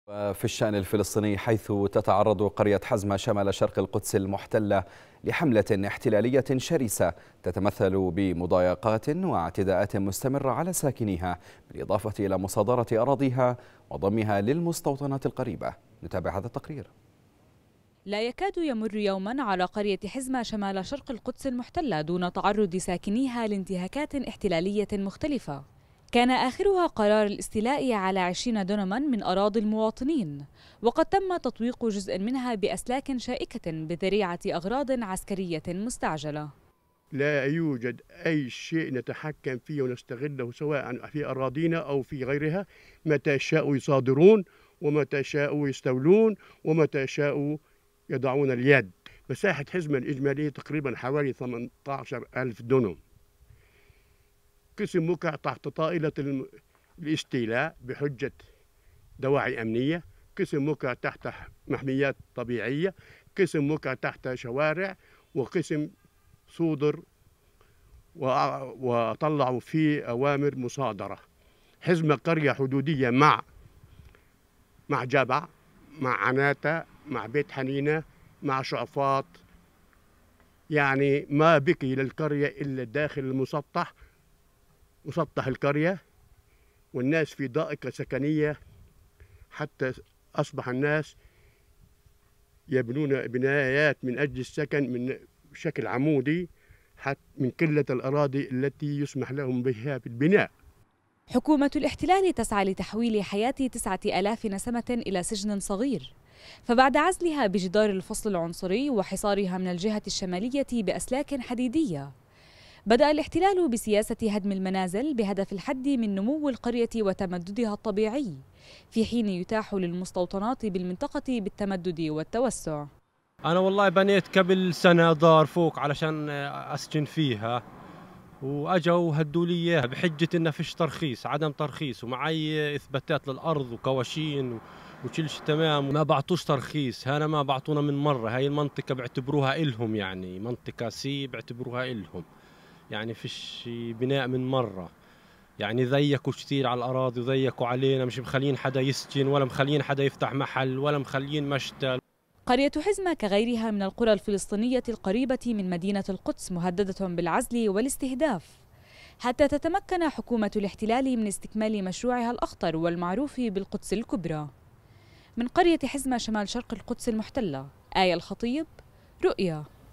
حملة مضايقات واعتداءات ومصادرة اراضي تتعرض لها قرية حزما - (5-11-2017)
تتعرضُ قرية حزما شمالَ شرقِ القدسِ المحتلةِ لحملةٍ احتلاليةٍ شرسة تتمثلُ بمضايقات واعتداءاتٍ مستمرةٍ على ساكنيها بالاضافةِ إلى مصادرةِ أراضيها وضمِها للمستوطناتِ القريبة.

تقرير: اية الخطيب.